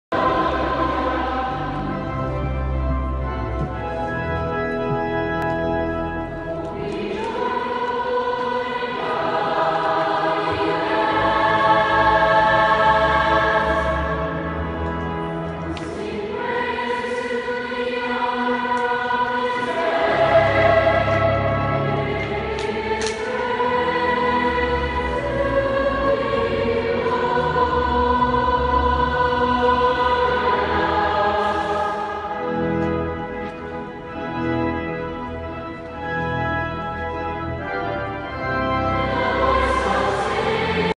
St Bonaventure Children's Chorale Pueri Cantores Music Festival St. Mary Cathedral Miami
St. Bonaventure Children's Chorale from Davie, Florida participated at the American Federation Pueri Cantores at St. Mary's Cathedral in Miami, February 25, 2017